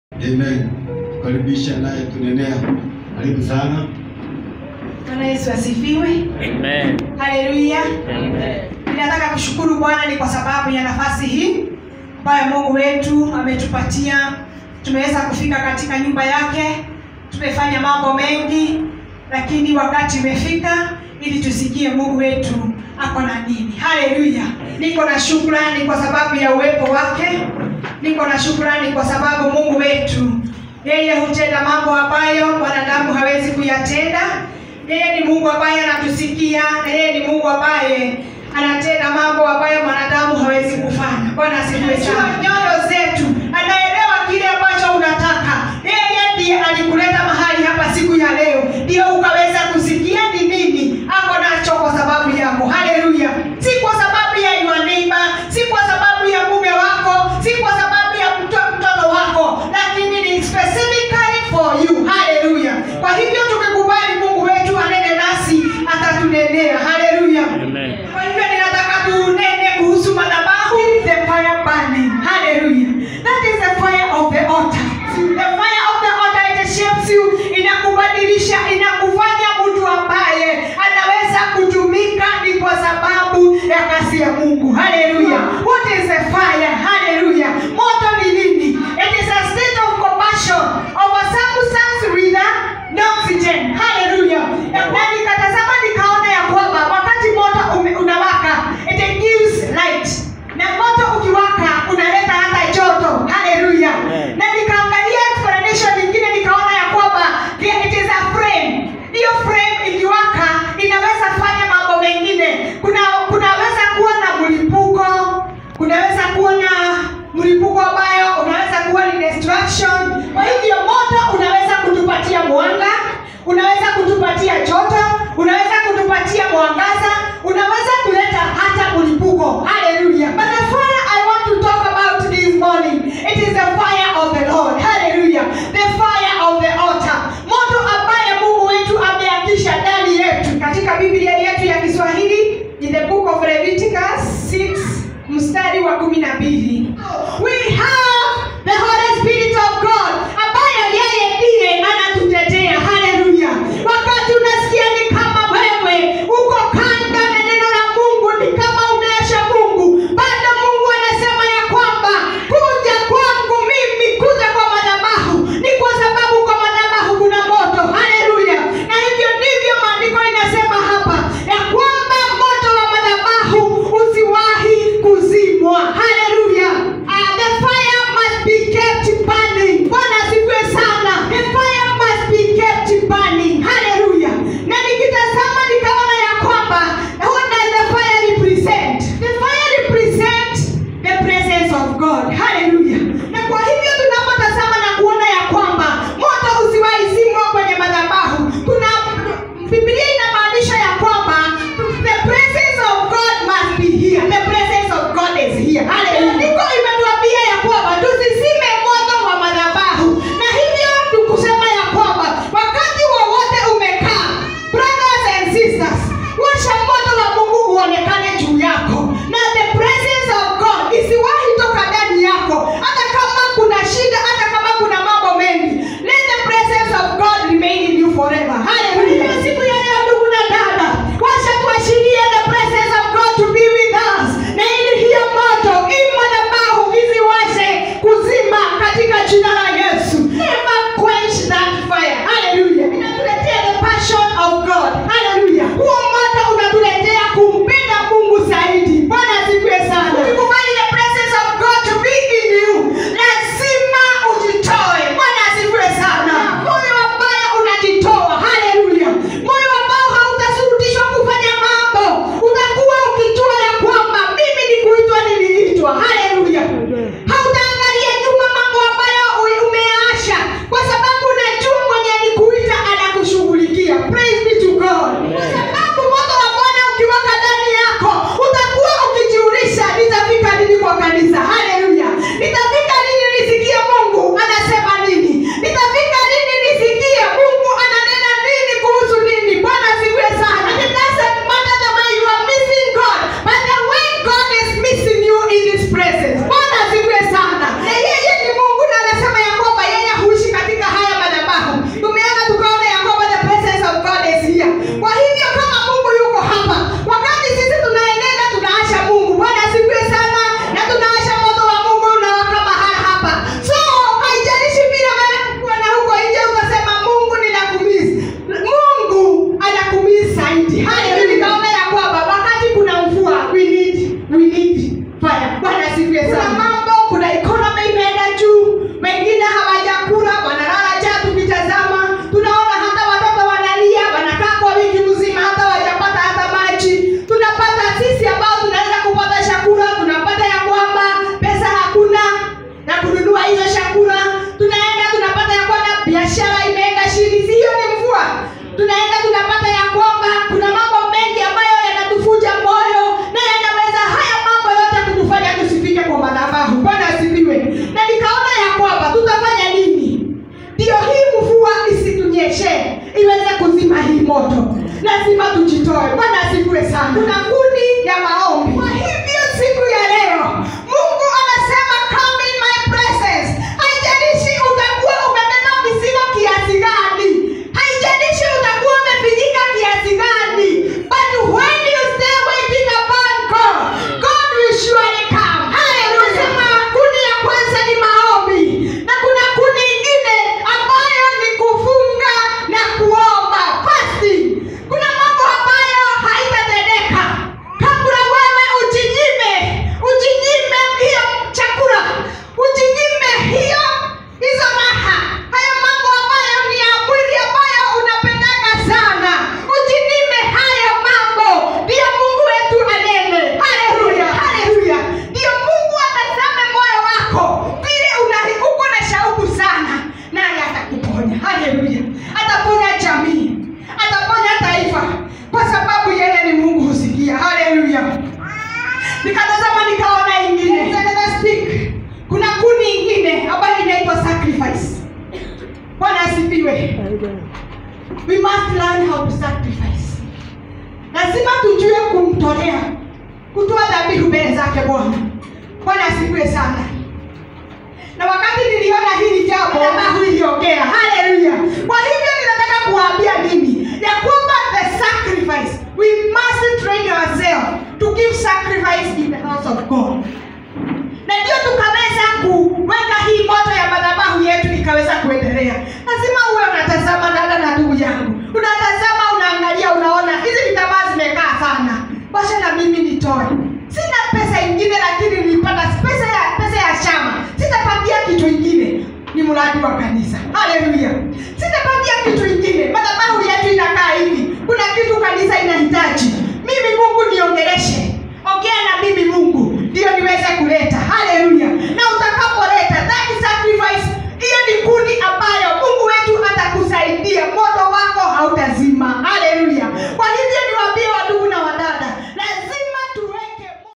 In the Presence of the Alter of GOD Message by Mrs Rev Lydia Maina/ WGCK/ Pangani Nairobi
WGCK